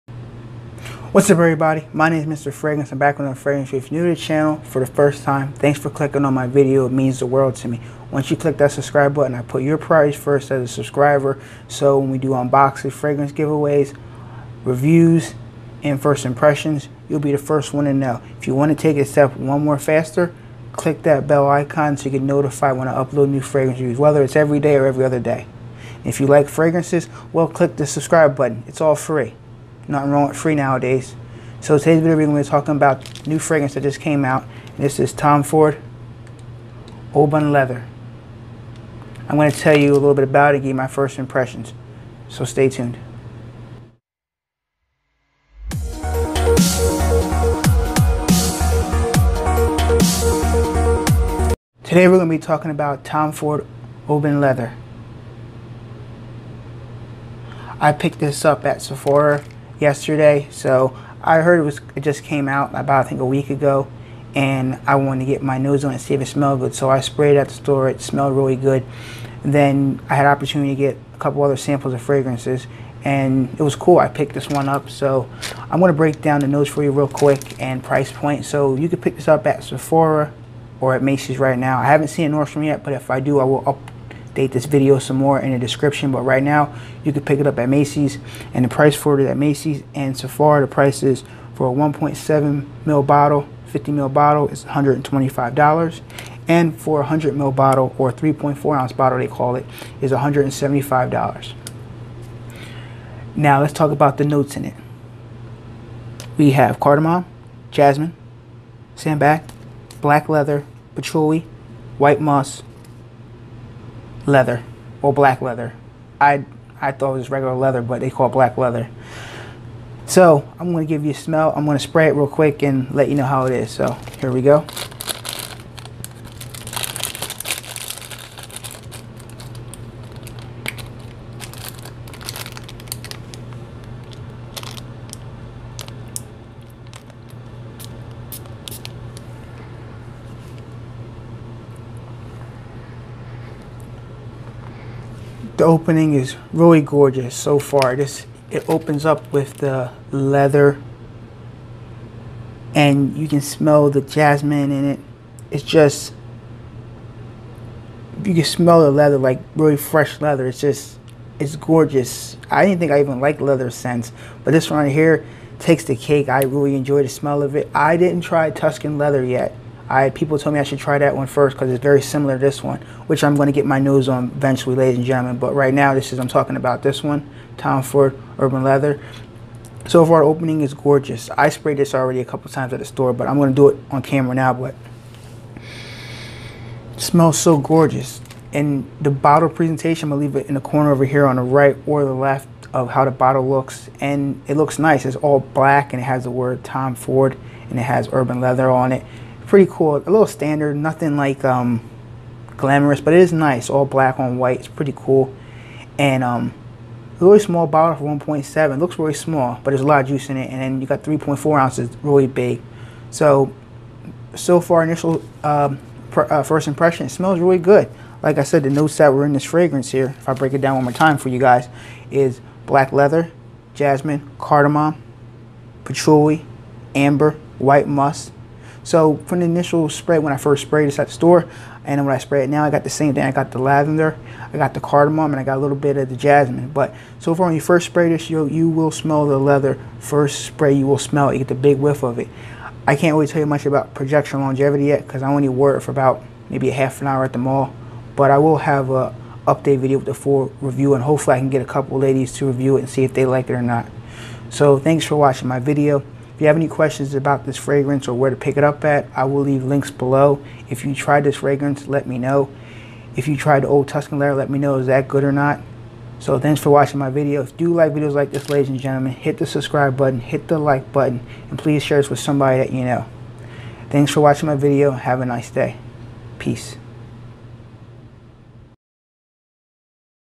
Tom Ford Ombr'e leather/ first impression
Welcome everyone to my channel Undercover fragrances. I'm Mr.Fragranes and today we are going talk about New Tom Ford Omb're leather. I hope everyone has a great day today. Until next time ladies and gentlemen goodbye.

Price $125- 1.7oz bottle and $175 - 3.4 oz bottle
Buy at Macy's or sephora stores.

  *Business/promotions *
Fragrances reviews & Rates